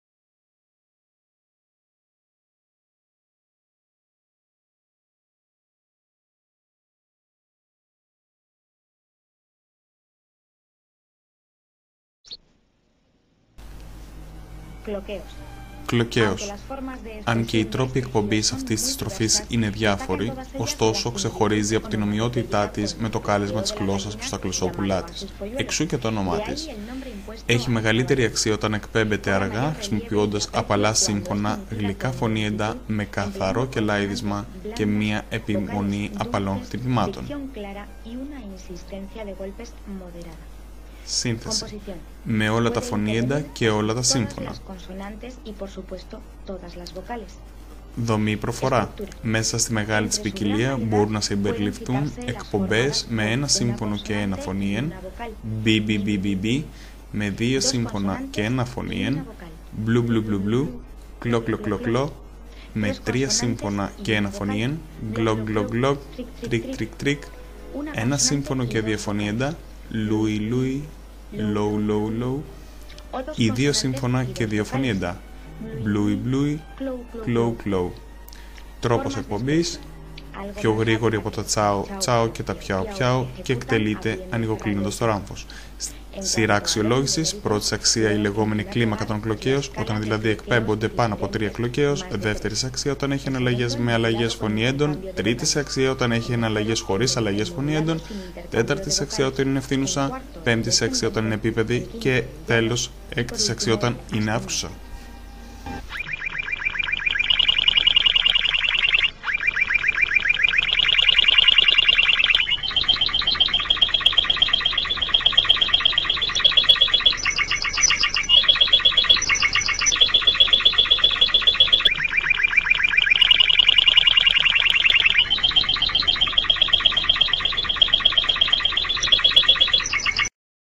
Cloqueos
Παρακολουθήστε μεταγλωττισμένο στα Ελληνικά από την ΕΛΙΤ βίντεο που αναφέρεται στην νότα "Cloqueos" του Ισπανικού καναρινιού φωνής Timbrado.